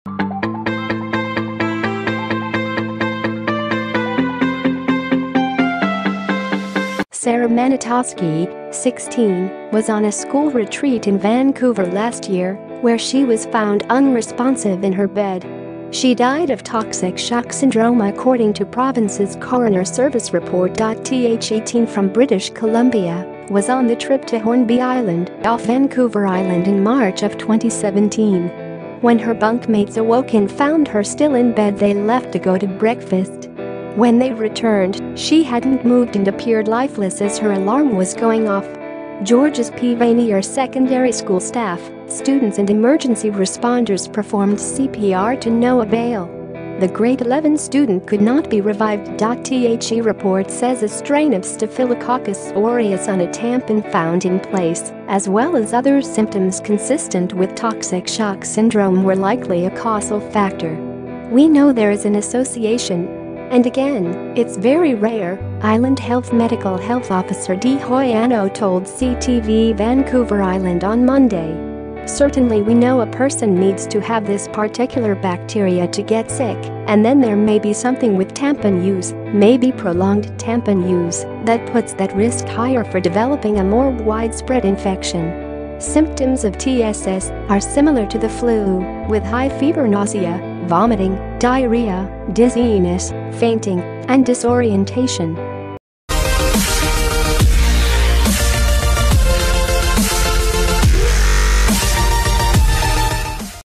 Teen on overnight school trip dies of toxic shock syndrome - 247 news
Teen,overnight,school,trip,dies,toxic,shock,syndrome
 Sara Manitoski, 16, was on a school retreat in Vancouver last year where she was found unresponsive in her bed. She died of toxic shock syndrome according to province's coroner service report.The teen from British Columbia was on the trip to Hornby Island, off Vancouver Island in March of 2017. When her bunk-mates awoke and found her still in bed they left to go to breakfast.When they returned, she hadn't moved and appeared lifeless as her alarm was going off.        Georges P. Vanier Secondary School staff, students and emergency responders performed CPR to no avail.The grade 11 student could not be revived.The report says a strain of staphylococcus aureus on a tampon found in place, as well as other symptoms consistent with toxic shock syndrome were likely a causal factor.  'We know there is an association. And again, it's very rare,' Island Health Medical Health Officer Dee Hoyano told CTV Vancouver Island on Monday. 'Certainly we know a person needs to have this particular bacteria to get sick, and then there may be something with tampon use, maybe prolonged tampon use, that puts that risk higher for developing a more widespread infection.' Symptoms of TSS are similar to the flu, with high fever nausea, vomiting, diarrhea, dizziness, fainting, and disorientation.